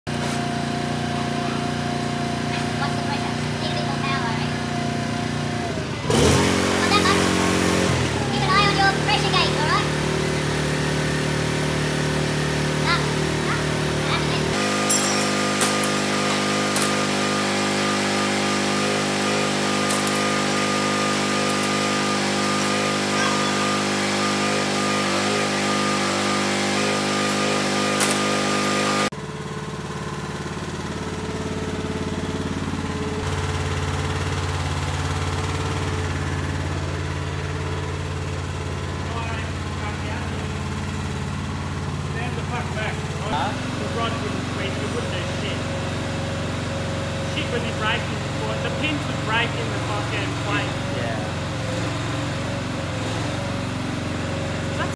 CYLINDER BREAKOUT BENCH EARNS ITS FIRST FEW DOLLARS
After 3 weeks of Cutting, Welding and Testing the torque tube cylinder breakout bench here is a couple of brief Videos of Seth and I using it on a real paying job.

First, we used the red heavy-duty 3-inch bore cylinder on the Breakout bench to disassemble and loosen a tight piston-nut offa chrome Rod .

The second short clip shows me tightening up the piston nut onto the chromed rod using the black 2-1/2 inch bore cylinder.

Using basic mathematics, pressure over area calculations and the distance of the cylinders point force from the centre pivot point, we worked out how much pressure we should use to tighten the piston-nut with a 60 mm dia thread up to 6920 Nm (5100 Foot-pounds).